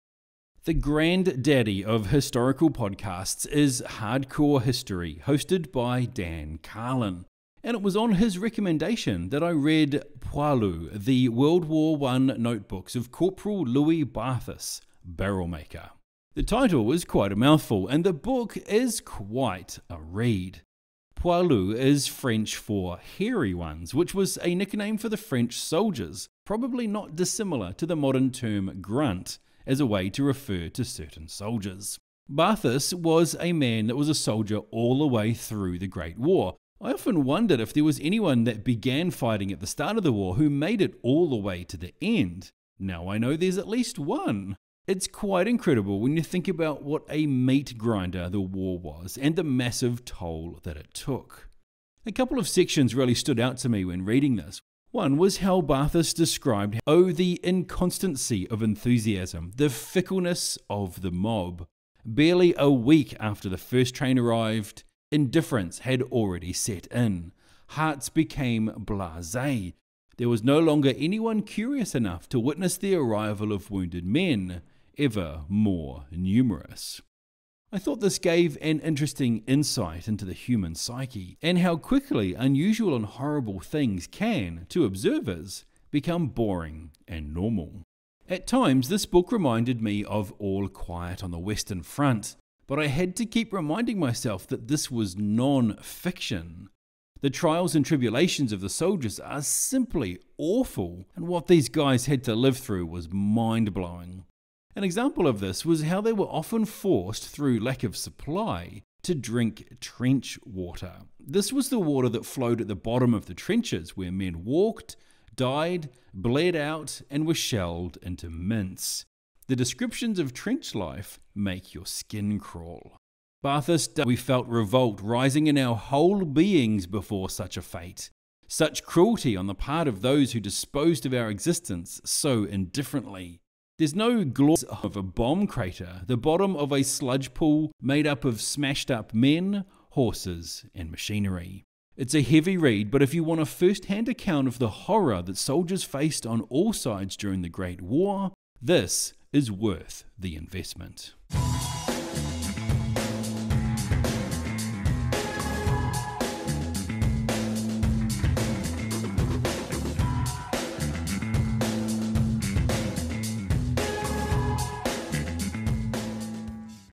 BOOK REVIEW | Poilu - The World War I Notebooks of Corporal Louis Barthas (Louis Barthas)
The one with the book that takes you into the mud filled trenches of The Great War...

If you notice a skip in this video, that's because YouTube made me cut some content for copyright reasons.